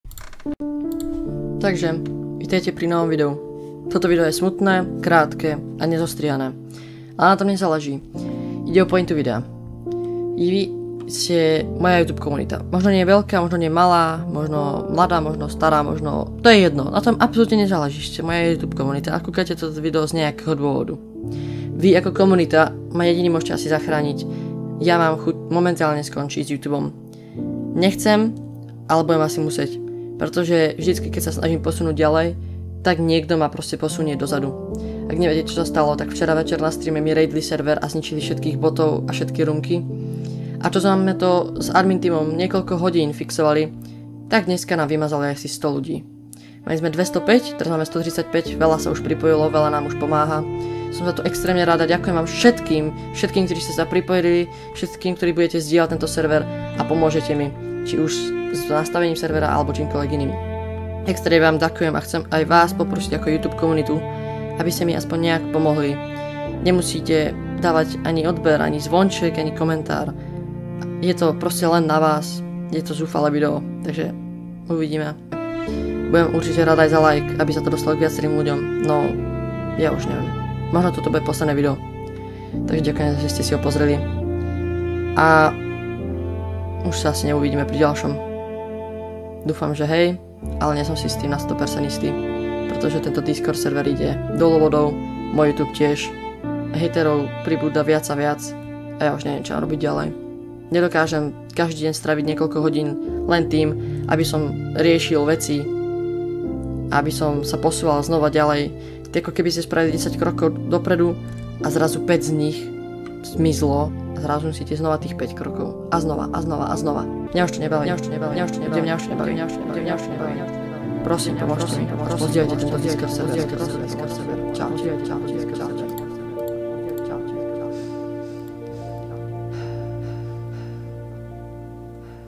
končím s Youtubom??? (niekto mi zničil celí discord) **upozornenie** sad video
takže áno toto není clickbait naozaj sa rozhodujem že skončím dúfam že to nejako zvládnem no nič nesľubujem no nekončím len kvôli discordu je toho viac ale to už sú osobné veci. ak ma chceš podporiť určite mi môžeš dať like odber ale najviac mi pomôže keď pozdielaš môj discord